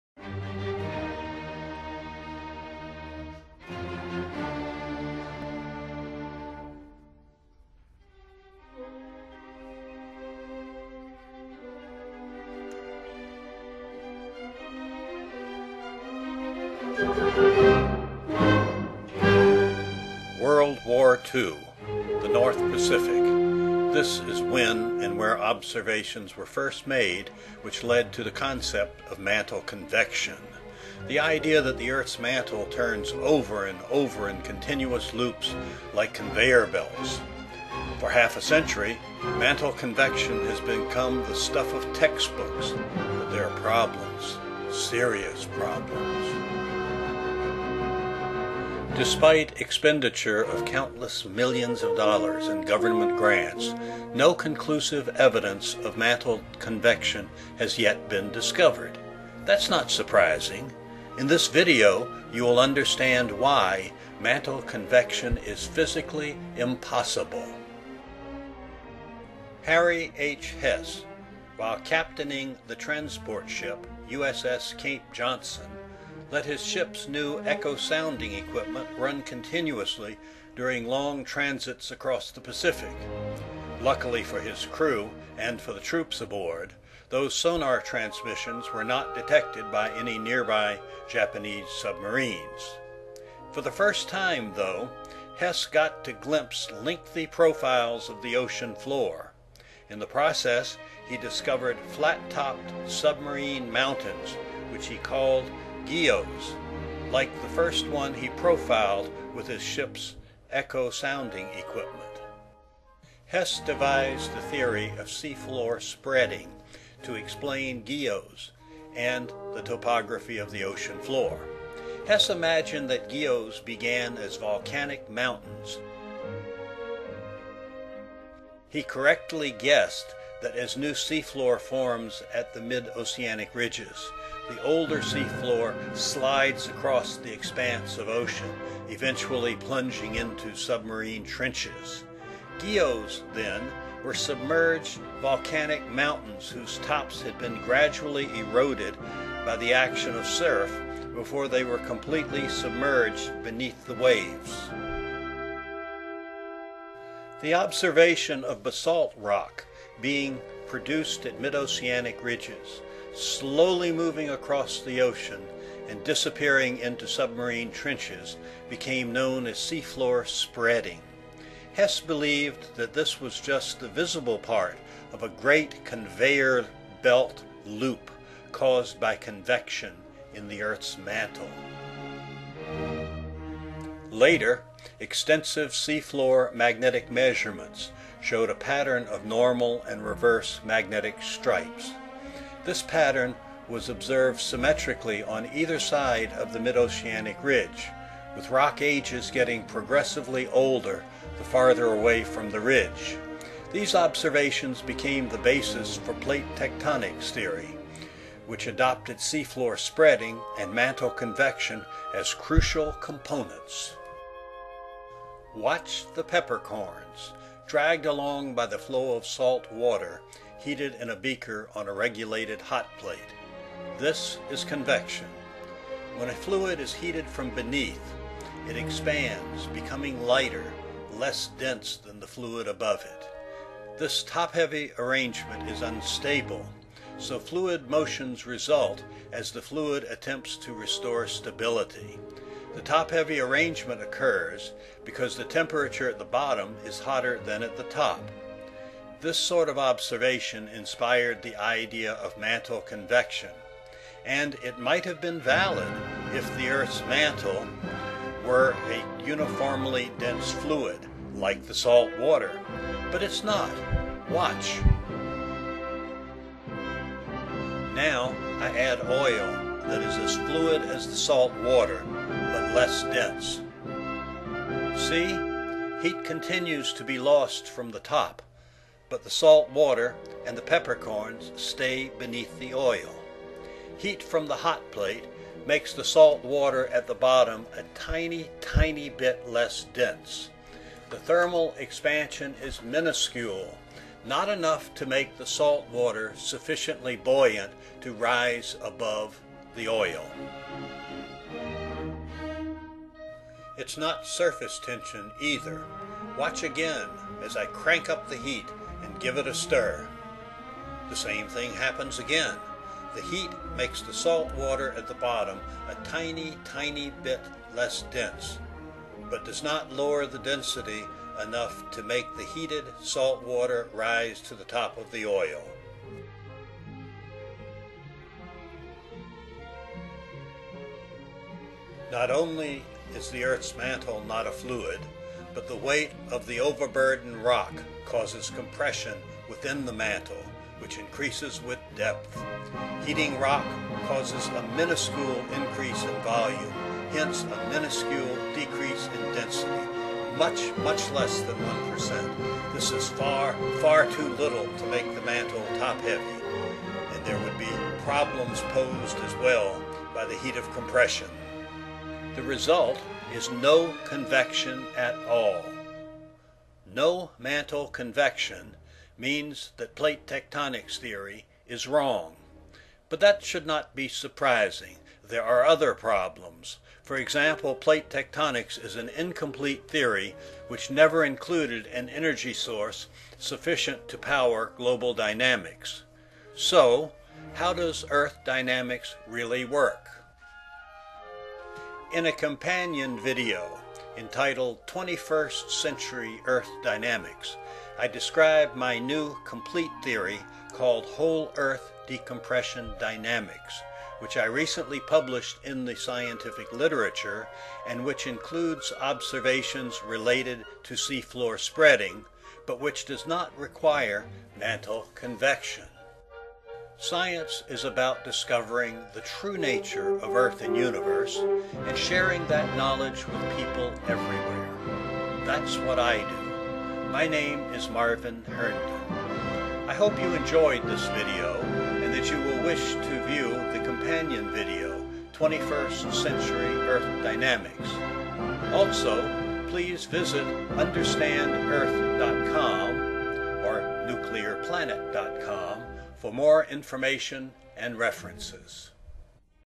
No Mantle Convection!!!
Plate tectonics theory is incomplete, providing no energy source for geodynamics and depends critically on the assumption of mantle convection. This video shows the reason that mantle convection is physically impossible. The companion video "21st Century Earth Dynamics" describes a new, complete theory of Earth dynamics called Whole-Earth Decompression Dynamics which does not depend upon mantle convection.